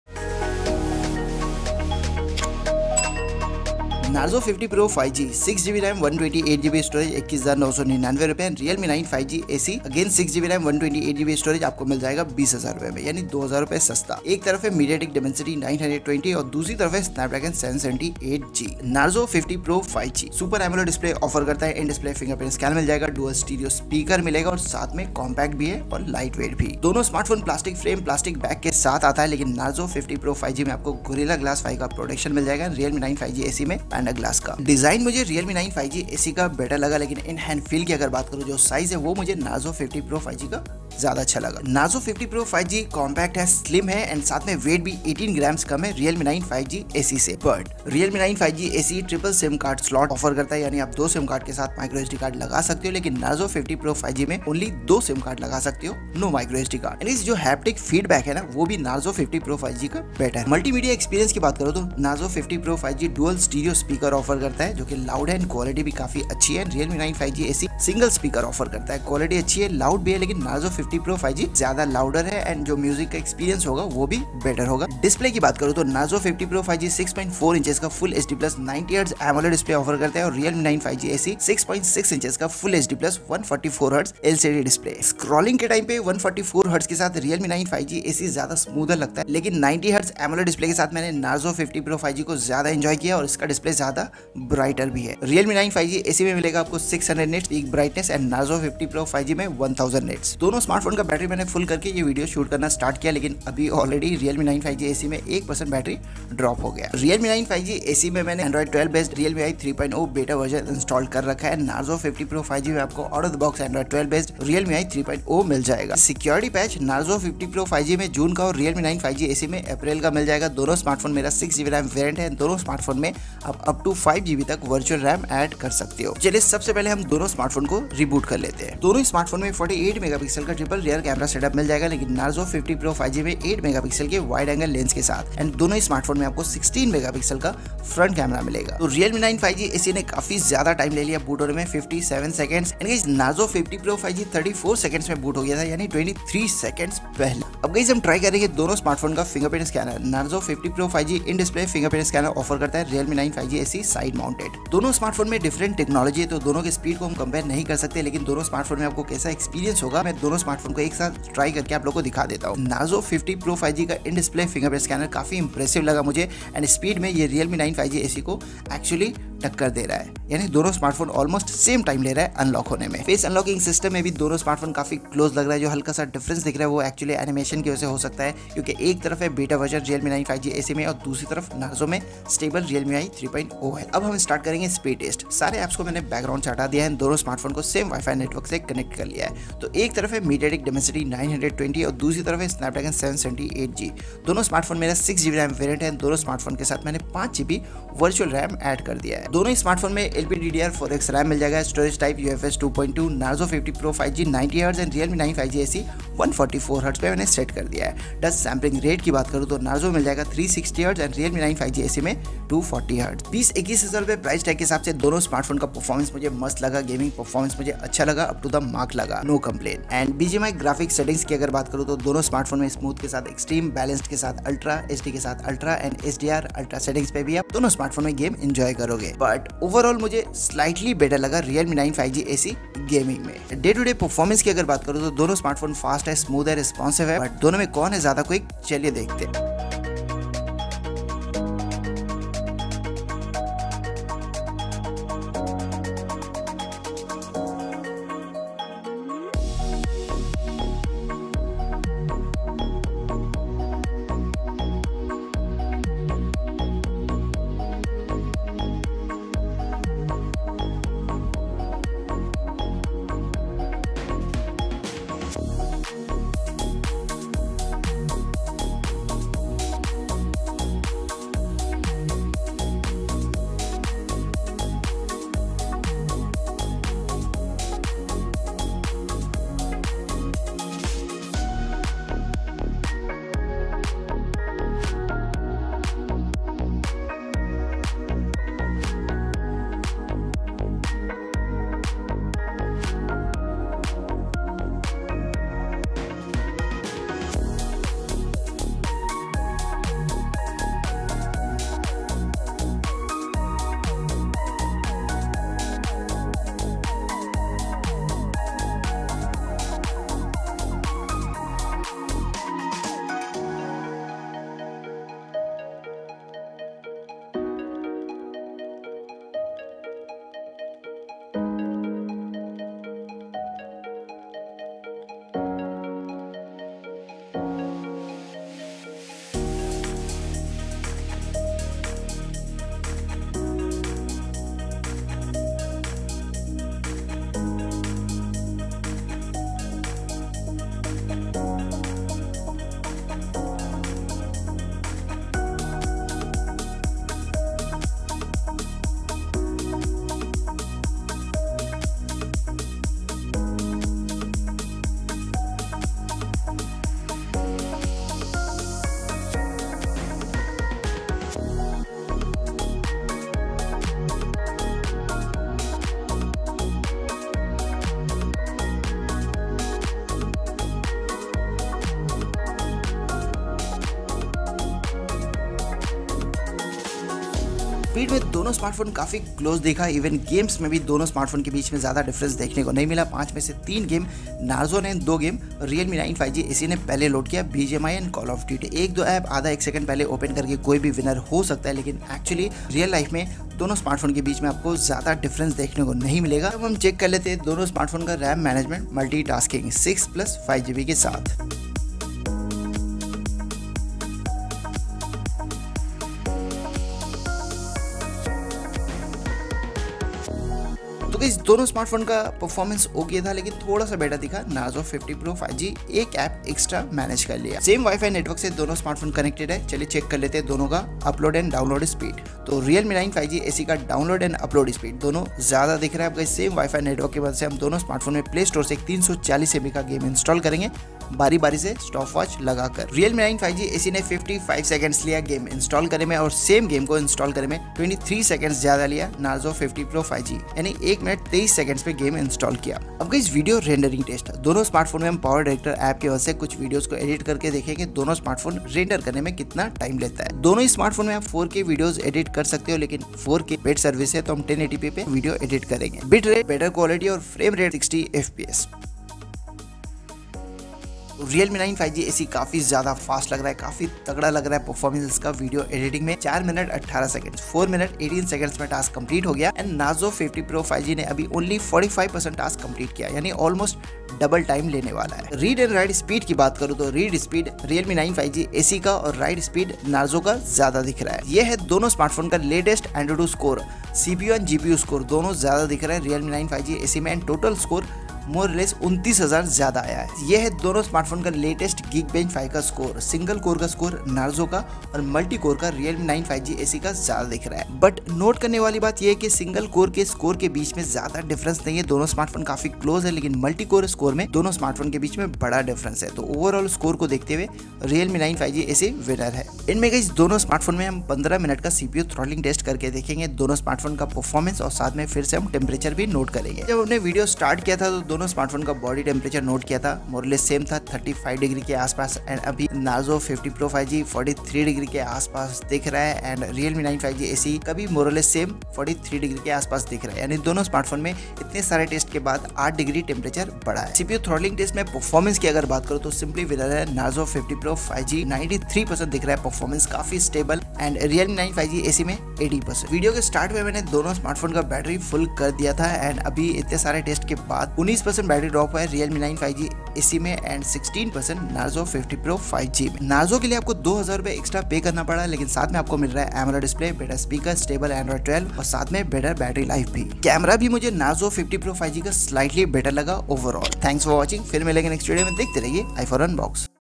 Realme 9 5G SE (Realme UI 3.0) vs Narzo 50 Pro 5G Performance and Speed Test.
Narzo 50 Pro 5G vs Realme 9 5G SE Speed Test.

Welcome to i4UNBOX, We brings you quality tech videos, unboxing, comparisons, tips and tricks and tech reviews regularly.